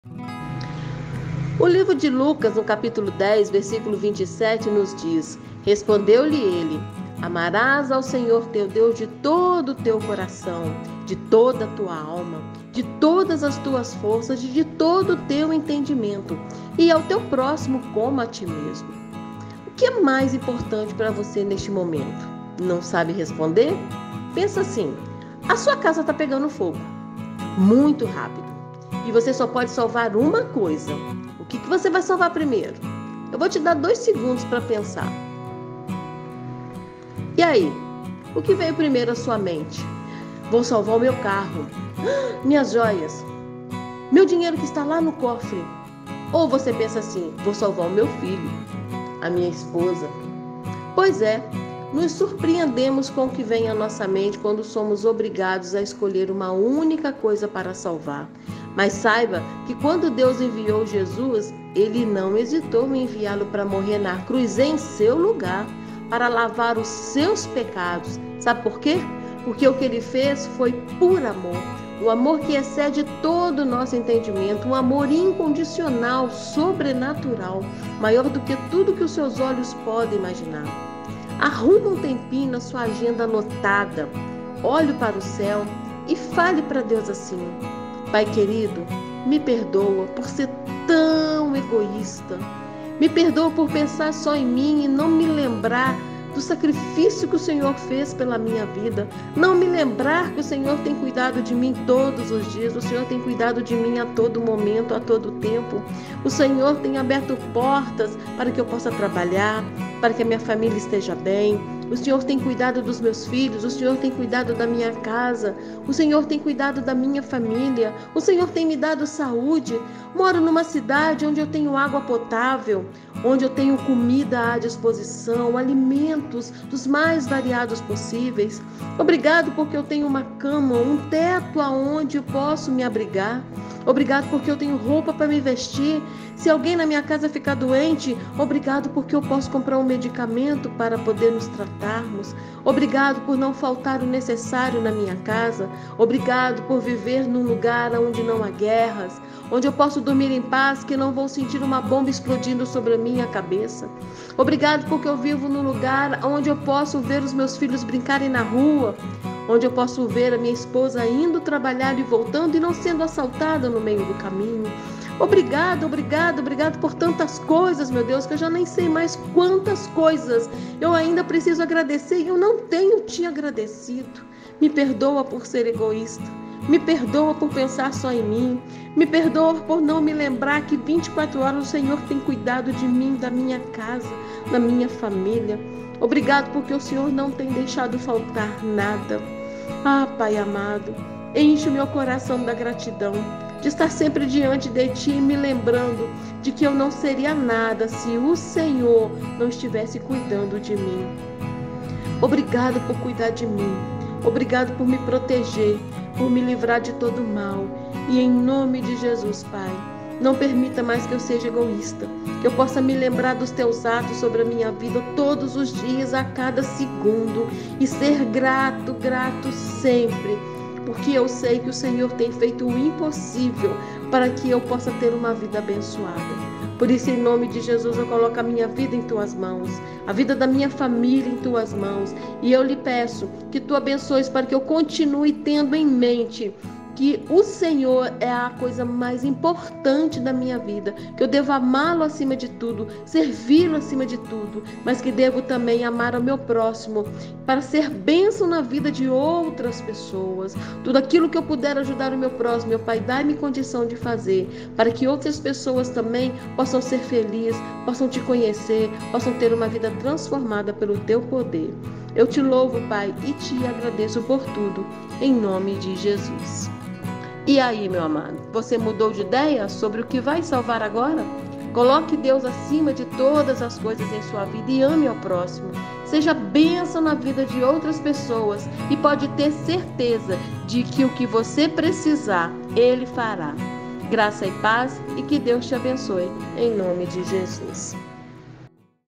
Amarás a Deus sobre todas as coisas..
O livro de Lucas 10:27 nos diz: Respondeu-lhe ele: Amarás ao Senhor teu Deus de todo o teu coração, de toda a tua alma, de todas as tuas forças e de todo o teu entendimento, e ao teu próximo como a ti mesmo.
O que é mais importante para você neste momento?
Coloque Deus acima de todas as coisas em sua vida, e ame ao próximo, seja bênção na vida de outras pessoas e pode ter certeza de que o que você precisar Ele fará.
E não se esqueça “Se tu podes crer, tudo é possível ao que crê” (Marcos 9:23)
Elisângela Braga